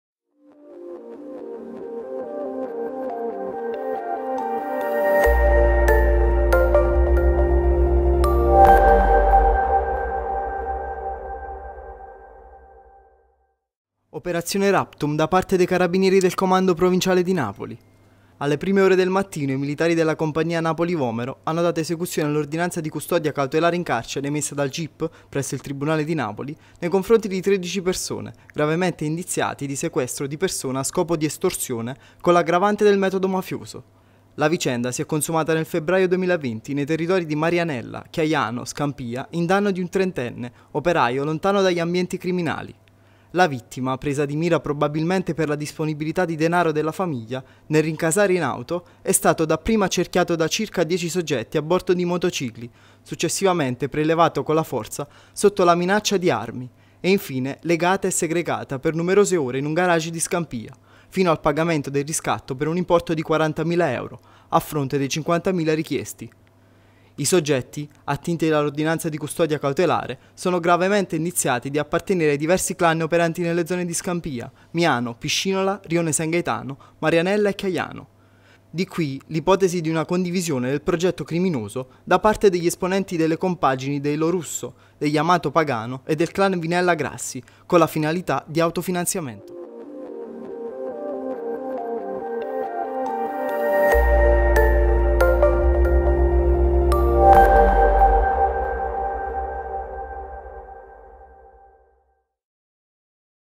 Napoli, patto tra clan per sequestro di persona: 13 arresti
Operazione “Raptum” da parte dei Carabinieri del Comando Provinciale di Napoli.

Alle prime ore del mattino, i militari della Compagnia Napoli Vomero, hanno dato esecuzione all’ordinanza di custodia cautelare in carcere emessa dal G.I.P. presso il Tribunale di Napoli nei confronti di 13 soggetti, gravemente indiziati di sequestro di persona a scopo di estorsione con l’aggravante del metodo mafioso.

La vicenda si è consumata nel febbraio 2020 nei territori di Marianella, Chiaiano, Scampia in danno di un 30enne, operaio lontano dagli ambienti criminali.

La vittima, presa di mira probabilmente per la disponibilità di denaro della famiglia, nel rincasare in auto, è stato dapprima accerchiata da circa 10 soggetti a bordo di motocicli, successivamente prelevata con la forza sotto la minaccia di armi e infine legata e segregata per numerose ore in un garage di Scampia, fino al pagamento del riscatto per un importo di 40mila euro, a fronte dei 50mila richiesti.

I soggetti attinti dalla ordinanza di custodia cautelare sono gravemente indiziati di appartenere ai diversi clan operanti nelle zone di Scampia, Miano, Piscinola, Rione San Gaetano, Marianella e Chiaiano, di qui l’ipotesi di una condivisione del progetto criminoso da parte degli esponenti delle compagini dei Lo Russo, degli Amato - Pagano e del clan Vinella Grassi con la finalità di auto-finanziamento.